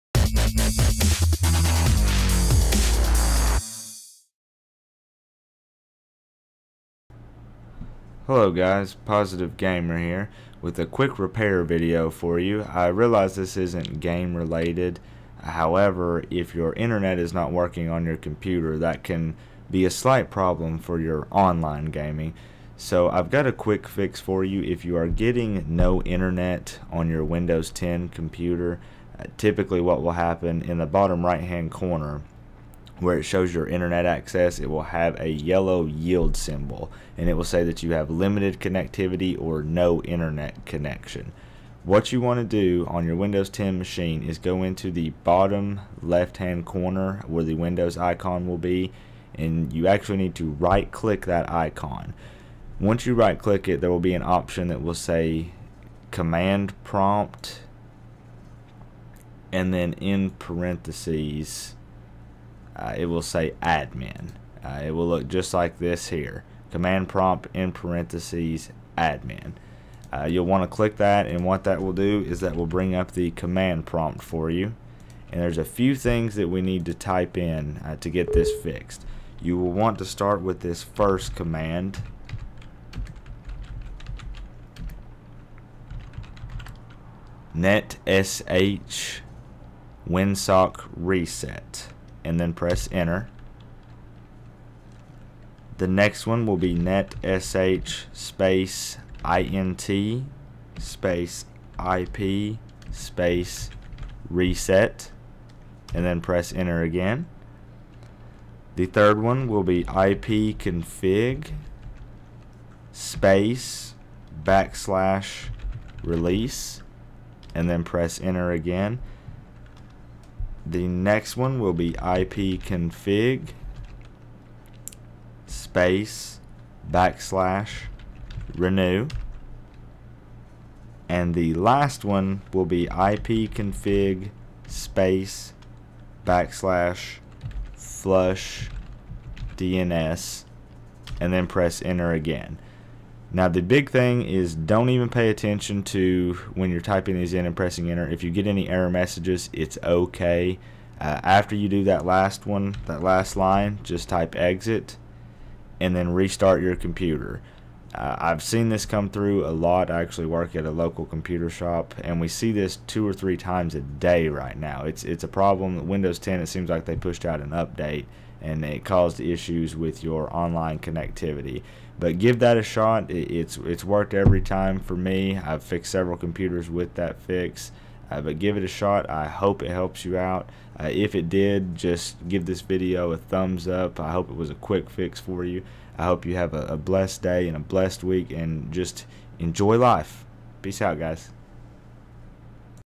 Fix Windows 10 No Internet in UNDER 2 MINUTES!
This is a quick tutorial on how to correct your Windows 10 PC that cannot connect to the internet. The text that you type into cmd prompt is as follows:

ipconfig /release

ipconfig /renew

ipconfig /flushdns